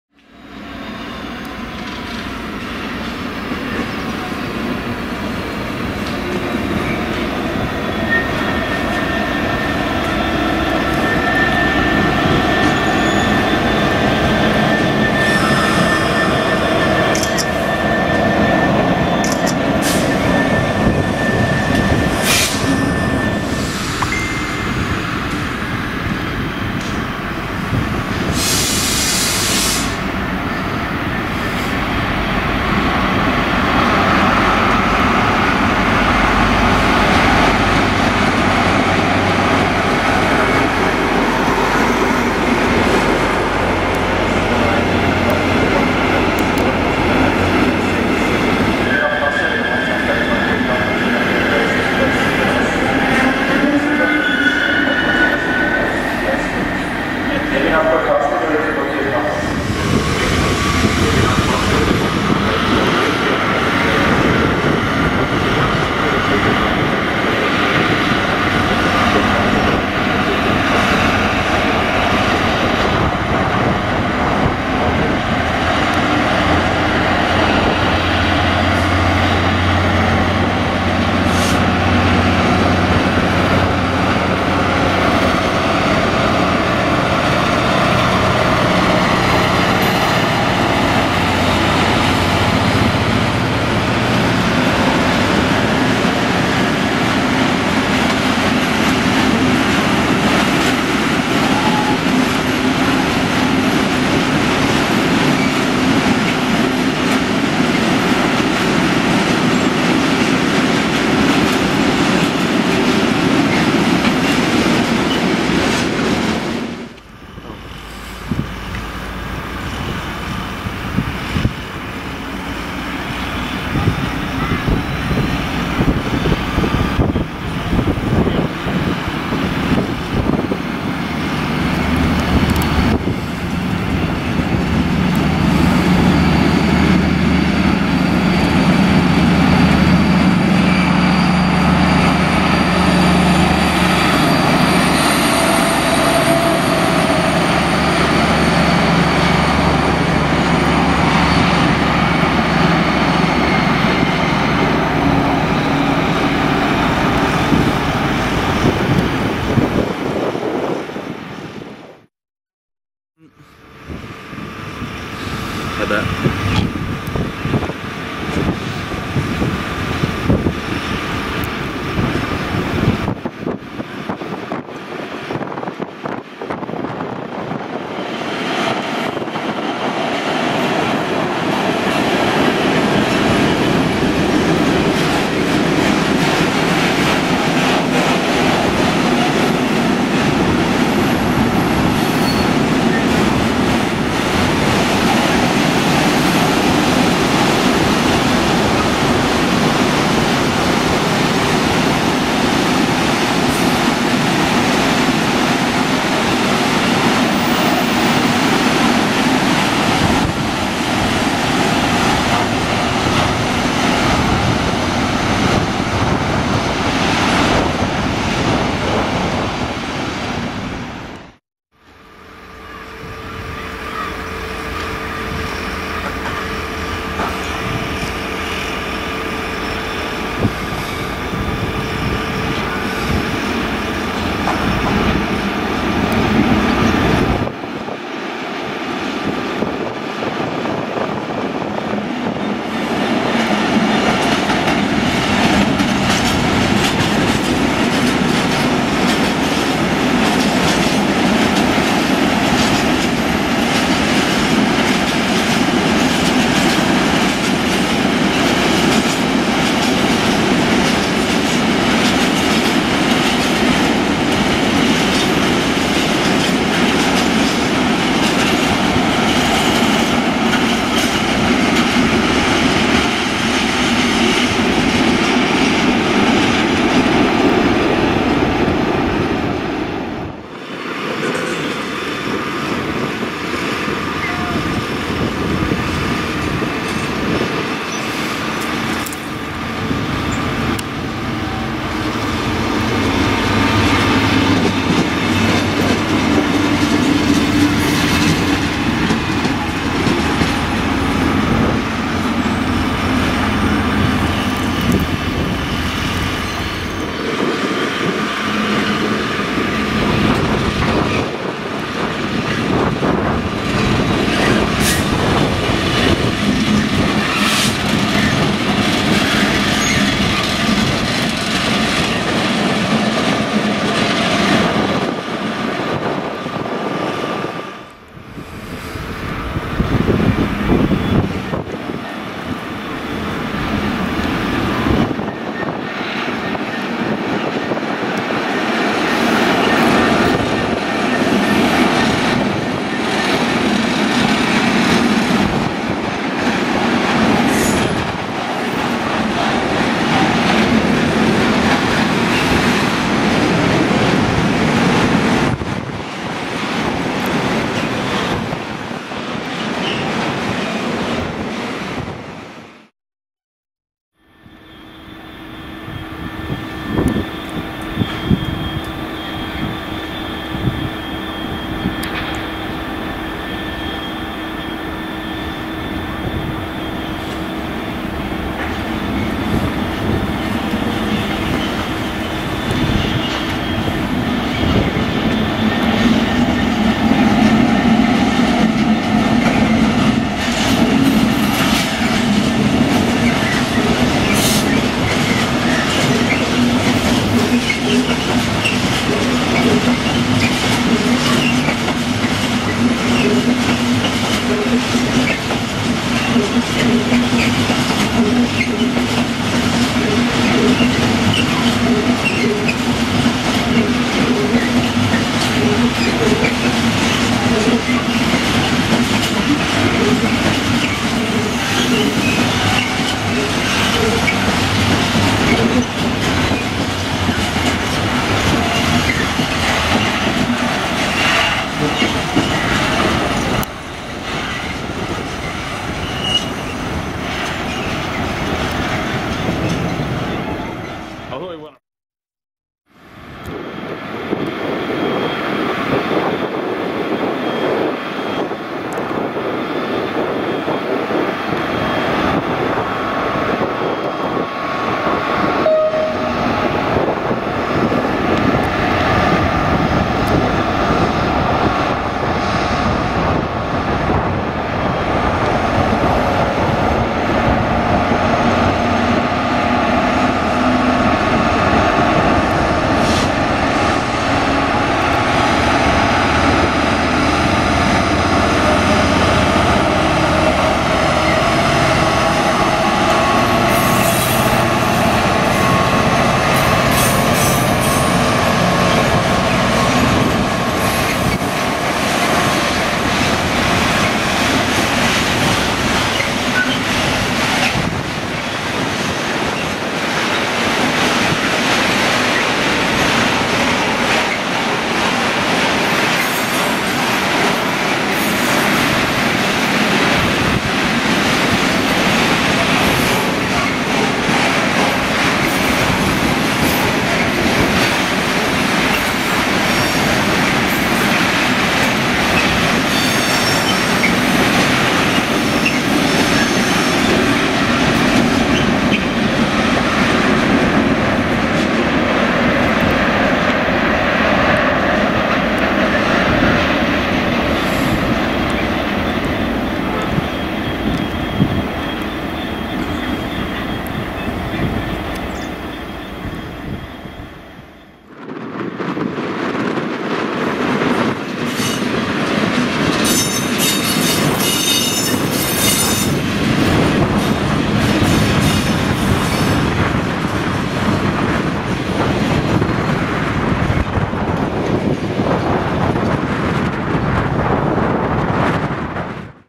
(HD) Trains at Mühldorf (Oberbay) with Euro Cargo Rail 247s - 23/6/15
Last day in Mühldorf (Oberbay) spent a few hours here before going back to Munich. In order we see:

1: A 247 051 shunts
2: A 218 430 arrives on Dosto stock from Munich
3: A 247 045 departs full thrash on a tanker train
4: 218 430 departs back for Munich
5: 247 043 arrives with a tanker train
6: Another 247 007 arrives with a tanker train
7: A 363 + 245 & 2 218's are shunted around the depot
8: 247 007 shunts light engine
9: A 247 arrives on a short tanker train
10: Another 247 038 arrives on another tanker train
11: A 247 055 departs on full thrash with a tanker train  a 232 on the rear
12: 294 xxx arrives on a binliner